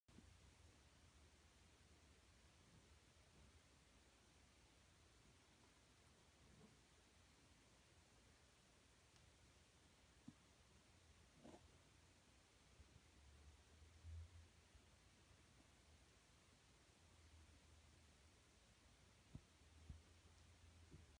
Russian Tortoise pees in bath.
Russian Tortoise pees in the bath tub.